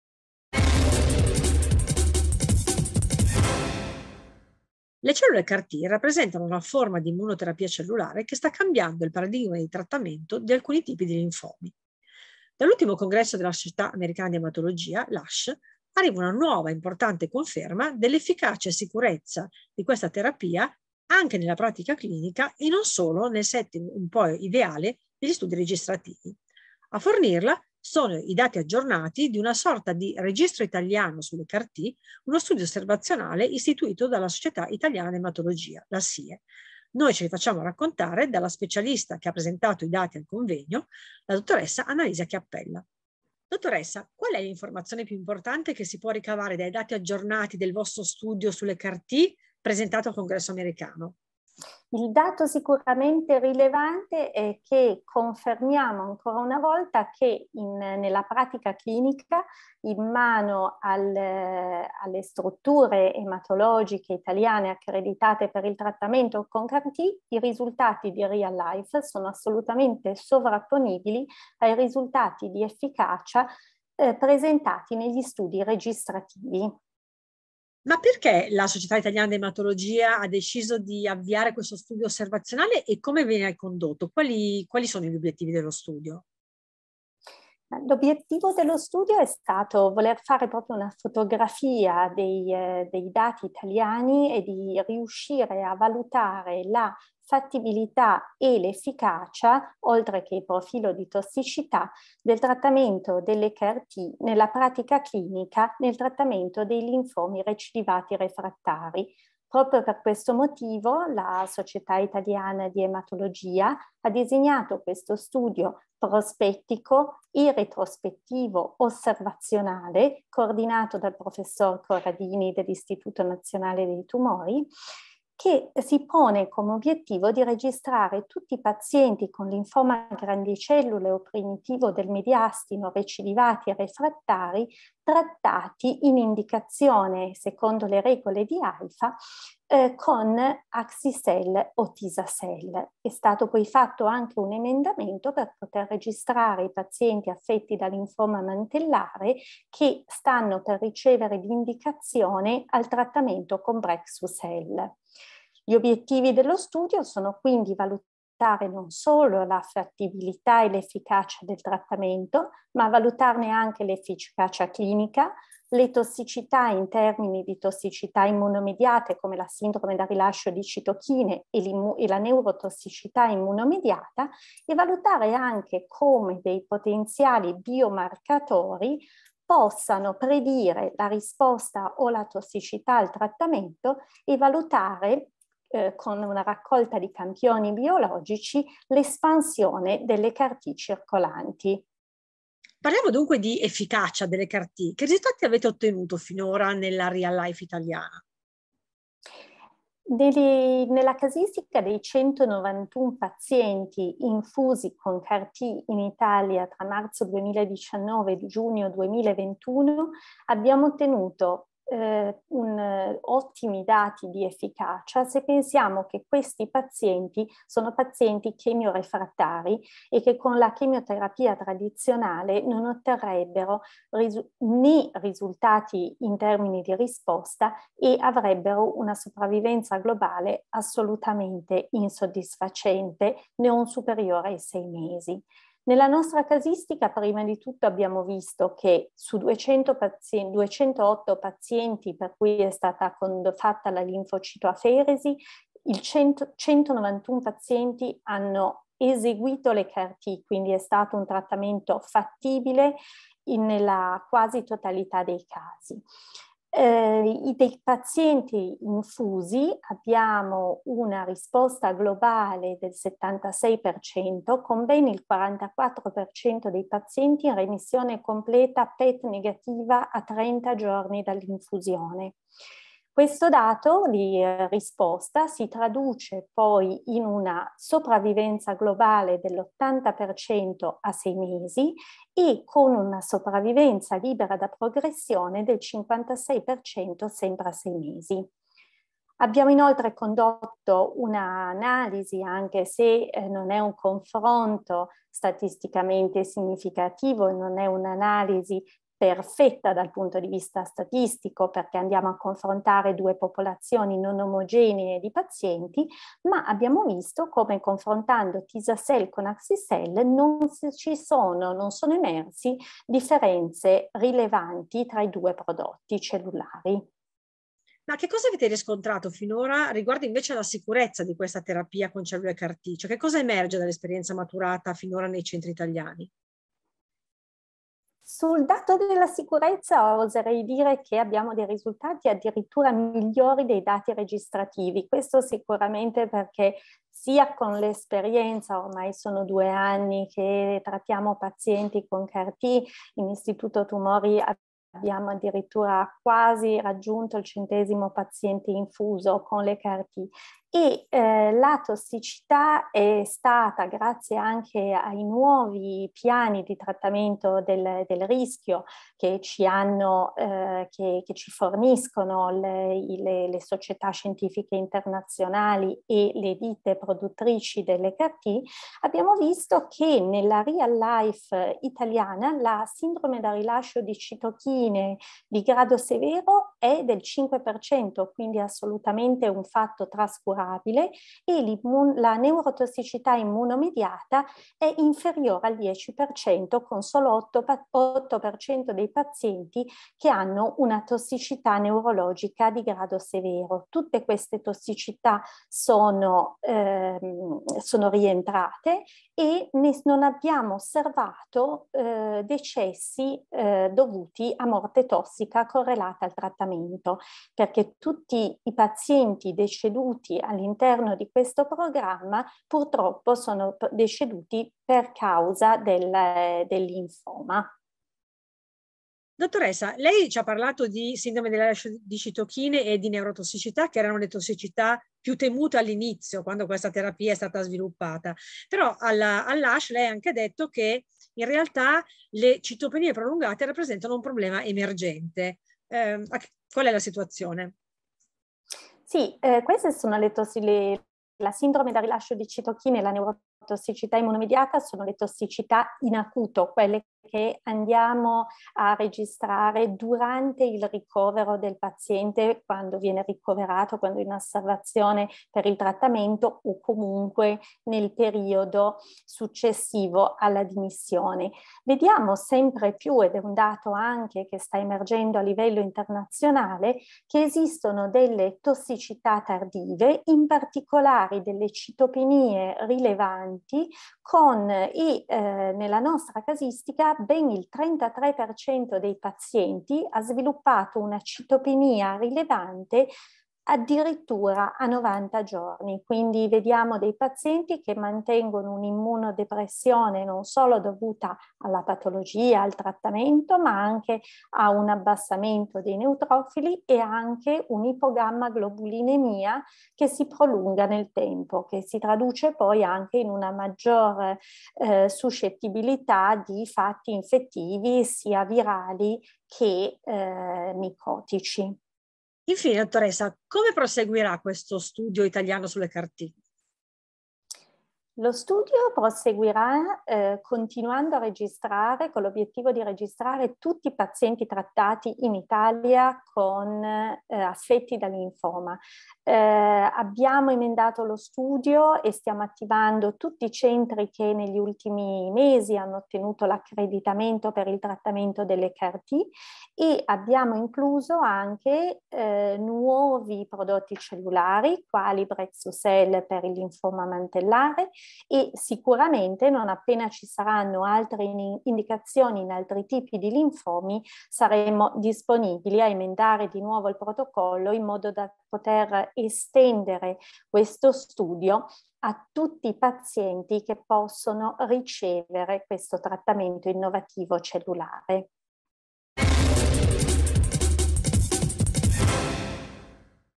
Linfomi a grandi cellule B, registro italiano CAR-T conferma efficacia e sicurezza nella real life
Le cellule CAR-T rappresentano una forma di immunoterapia cellulare che sta cambiando il paradigma di trattamento di alcuni tipi di linfomi. Dall’ultimo congresso dell’American Society of Hematology (ASH) arriva una nuova conferma dell'efficacia e sicurezza di questa terapia anche nella pratica clinica e non solo nel setting ideale degli studi registrativi. La conferma proviene dai dati aggiornati del registro italiano sulle cellule CAR-T, uno studio osservazionale istituito dalla Società Italiana di Ematologia (SIE). Ne parliamo in quest'intervista con la specialista che ha coordinato e presentato lo studio, la Dottoressa Annalisa Chiappella, Dirigente medico della Divisione di Ematologia della Fondazione IRCCS Istituto Nazionale dei Tumori di Milano.